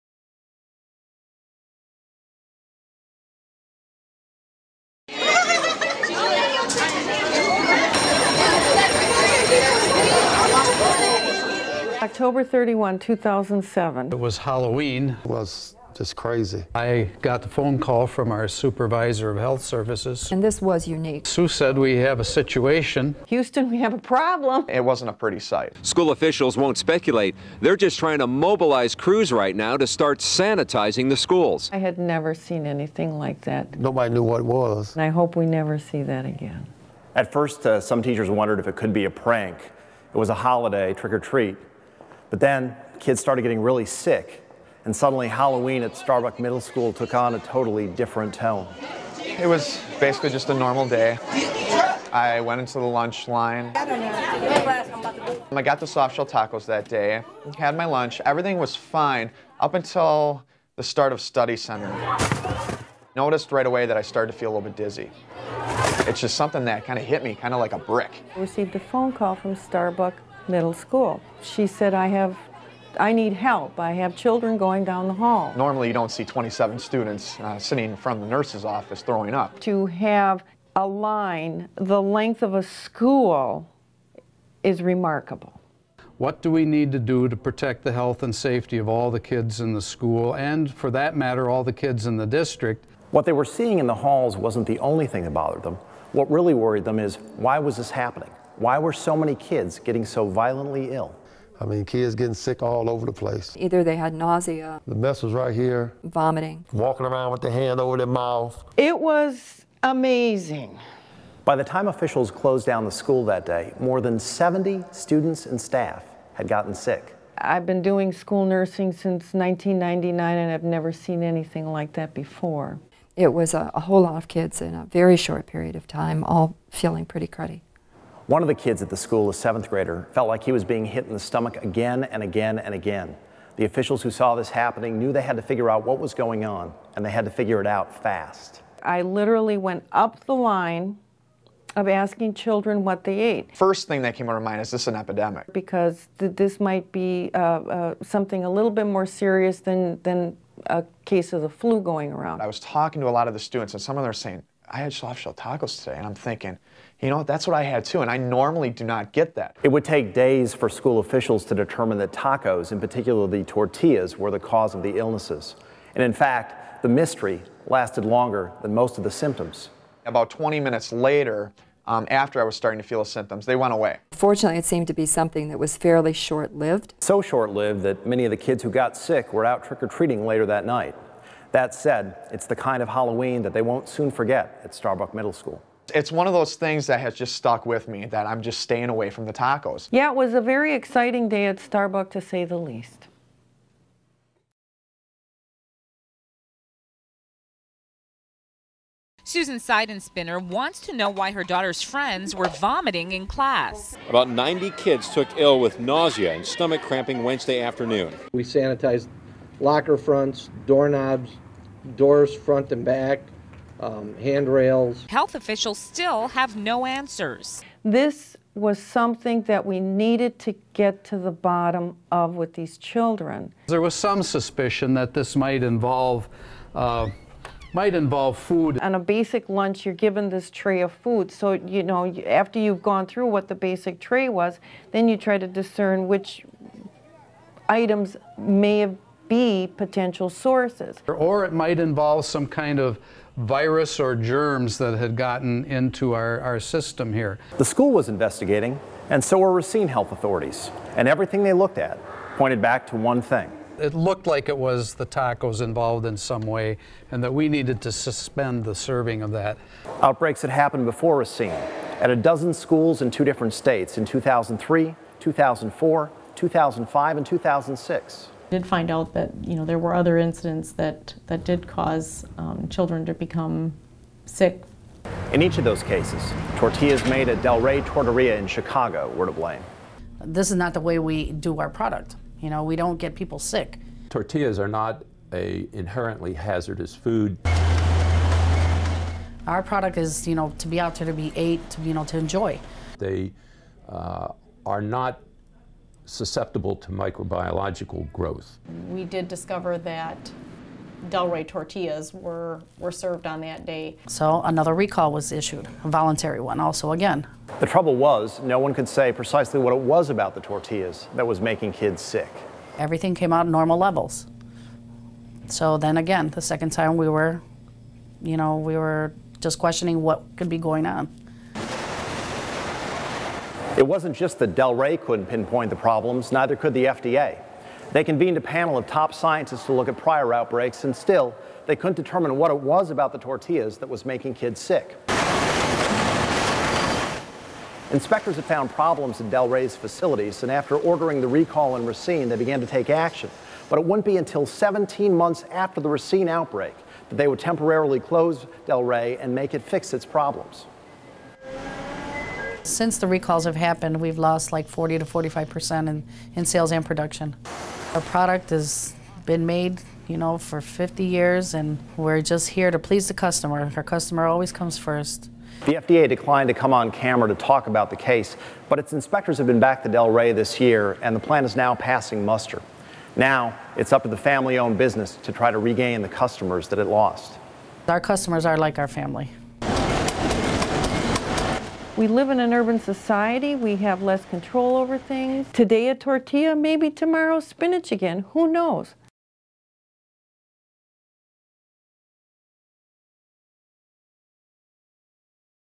School lunches: Trouble on the tray
Learn about what happened on taco day in Racine, Wis., that led to 101 students becoming ill on Halloween 2007.  Discover why it took outbreaks at more than 20 schools across the country and nearly six years for the federal  government to close down a Chicago tortilla company.  By Garrett Hubbard/USA TODAY